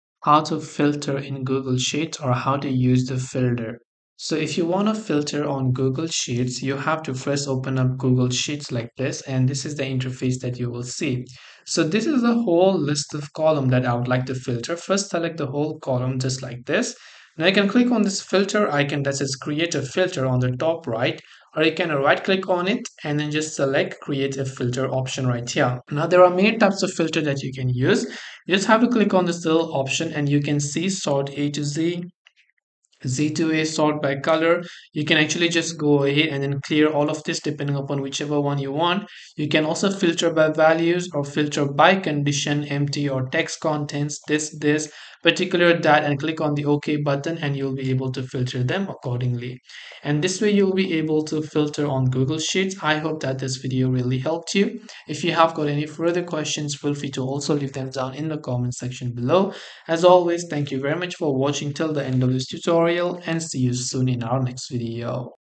How to Filter in Google Sheets !
Filtering in Google Sheets is a great way to view only the specific data you need, while hiding the rest. Whether you're working with large datasets or just want to find something quickly, filters help you sort and manage your data efficiently.

To begin using filters in Google Sheets, first open your document by navigating to sheets.google.com. Once inside your sheet, identify the column or range of data you want to filter. Select the entire column or range by clicking and dragging your mouse over the data you want to include in the filter. This could be a single column, multiple columns, or even the whole sheet depending on your needs.

Once you've selected the data, you'll notice a small icon at the top of the Google Sheets interface that looks like a funnel. This is the filter icon. Click on this funnel icon to create a filter for the selected data. If you prefer, you can also right-click on the selected data and choose the "Create a Filter" option from the context menu. This will apply a filter to the data you selected.

After applying the filter, you will see small drop-down arrows appear next to each column header. These arrows allow you to customize the filter for each specific column. When you click on one of these drop-down arrows, a menu will appear, giving you several options for filtering your data. You can choose to sort the data in ascending or descending order (A to Z or Z to A), filter by color if your cells are color-coded, or create custom filters based on values or conditions.

For example, if you only want to display rows where a specific column contains certain values, you can clear all other values and check only the ones you want to keep. You can also filter by conditions such as text that contains certain keywords or numbers that fall within a specific range. Once you've selected your filter criteria, click "OK" to apply the filter. The data that doesn't meet the filter criteria will be hidden from view, making it easier to focus on the information you need.

If at any point you want to remove or reset the filter, simply click on the filter icon again and choose "Remove Filter" to restore your sheet to its original view.

Google Sheets' filtering tool is a powerful feature that can help you manage and analyze your data more efficiently. Whether you're sorting data alphabetically, by numerical value, or using more complex conditions, filters can save you time and effort when working with large datasets.

Timestamps:

0:00 Introduction
0:05 Selecting Data to Filter
0:17 Creating a Filter
0:29 Using Sort and Filter Options
0:41 Customizing Filters by Values or Conditions
0:55 Applying and Removing Filters
1:01 Conclusion